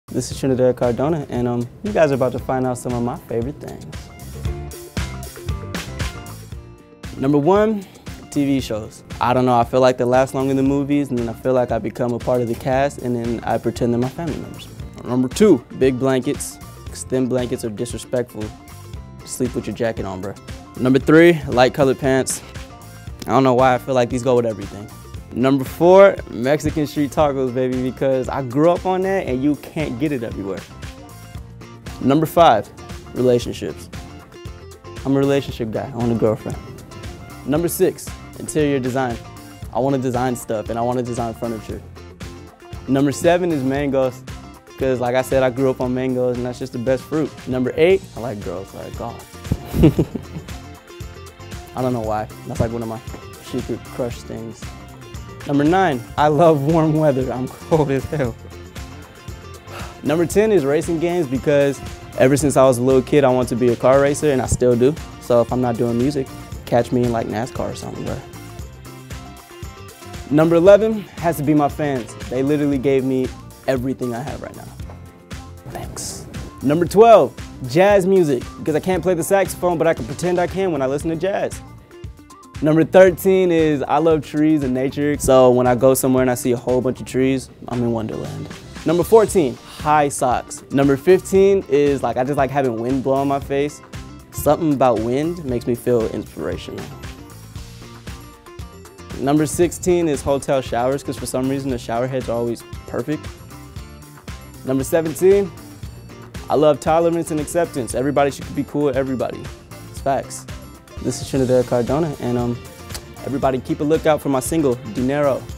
Trinidad Cardona | 17 Favorite Things
Trinidad Cardona has some favorites we can get down with!

Do you ever wonder if you and a celebrity have any weird habits in common? Did you ever google in the inside of an air conditioner just to see what it looks like?